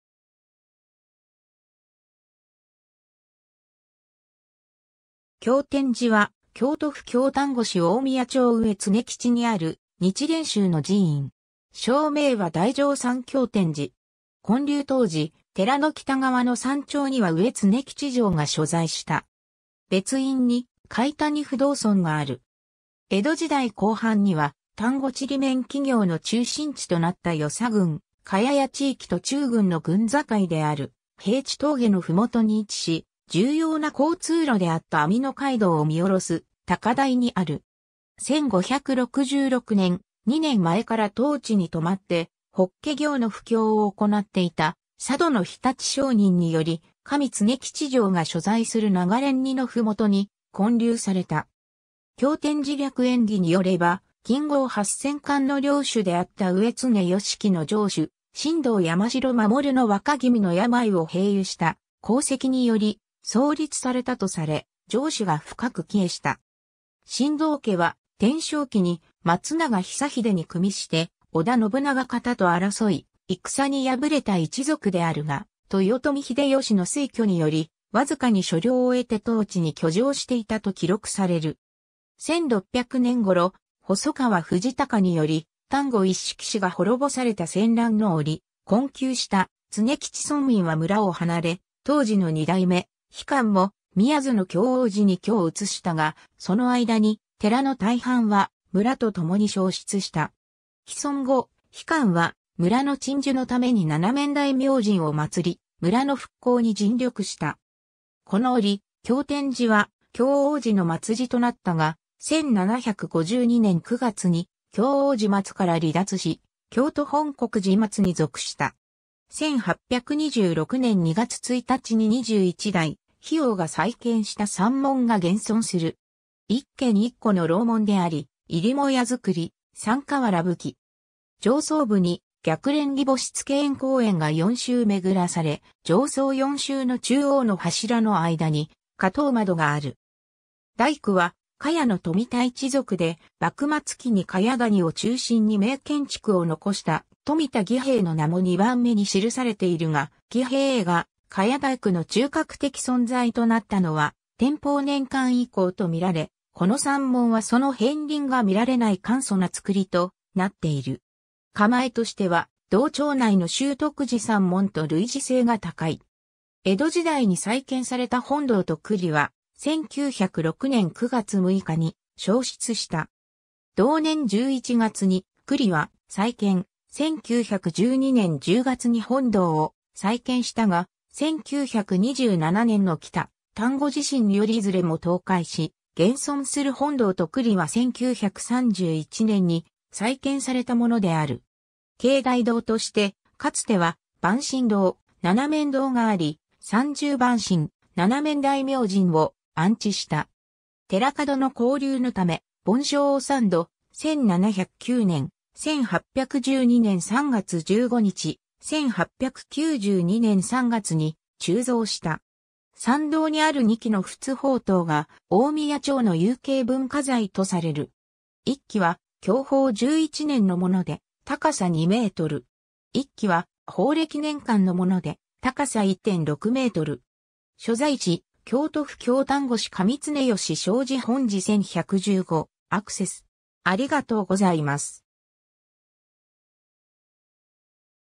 経典寺
経典寺（きょうてんじ）は、京都府京丹後市大宮町上常吉にある日蓮宗の寺院。
詳名は大乗山経典寺。
建立当時、寺の北側の山頂には上常吉城が所在した。
 別院に、貝谷不動尊がある。
 江戸時代後半には丹後ちりめん機業の中心地となった与謝郡加悦谷地域と中郡（現在の京丹後市大宮町,峰山町）の郡境である平地峠の麓に位置し、重要な交通路であった網野街道を見下ろす高台にある。
 1566年（永禄9年）、2年前から当地に留まって法華経の布教を行っていた佐渡の日立（にちりゅう）上人により、上常吉城が所在する名賀連尼（ながれお）の麓に建立された。
「経典寺略縁起」によれば、近郷8000貫の領主であった上常吉城の城主・進藤山城守の若君の病を平癒した功績により、創立されたとされ、城主が深く帰依した。
進藤家は、天正期に松永久秀に組して織田信長方と争い、戦に敗れた一族であるが、豊臣秀吉の推挙により、わずかに所領を得て当地に居城していたと記録される。
 1600年（慶長5年）頃、細川藤孝により丹後一色氏が滅ぼされた戦乱の折、困窮した常吉村民は村を離れ、当寺の2代目・日観も宮津の経王寺に居を移したが、その間に寺の大半は村とともに焼失した。
帰村後、日観は村の鎮守のために七面大明神を祀り、村の復興に尽力した。
この折、経典寺は経王寺の末寺となったが、1752年（宝暦2年）9月に経王寺末から離脱し、京都本圀寺末に属した。
 1826年（文政9年）2月1日に21代・日応が再建した山門が現存する。
一間一戸の楼門であり、入母屋造、桟瓦葺き。
上層部に逆蓮擬宝珠付縁高縁が4周巡らされ、上層4周の中央の柱の間に火灯窓がある。
大工は加悦の富田一族で、幕末期に加悦谷を中心に名建築を残した富田儀兵衛の名も2番目に記されているが、儀兵衛が加悦大工の中核的存在となったのは天保年間以降とみられ、この山門はその片鱗がみられない簡素なつくりとなっている。
構えとしては、同町内の周徳寺山門と類似性が高い。
 江戸時代に再建された本堂と庫裡は、1906年（明治39年）9月6日に焼失した。
同年11月に庫裡は再建、1912年（大正元年）10月に本堂を再建したが、1927年（昭和2年）の北丹後地震によりいずれも倒壊し、現存する本堂と庫裡は1931年（昭和6年）に再建されたものである。
 境内堂として、かつては番神堂、七面堂があり、三十番神・七面大明神を安置した。
 寺門の興隆のため、梵鐘を3度、1709年（宝永6年）、1812年（文化9年）3月15日、1892年（明治25年）3月に鋳造した。
 参道にある2基の仏宝塔が、大宮町の有形文化財とされる。
1基は享保11年のもので、高さ2メートル。
1基は宝暦年間のもので高さ1.6メートル。
 所在地 : 京都府京丹後市上常吉小字本地1115 アクセス :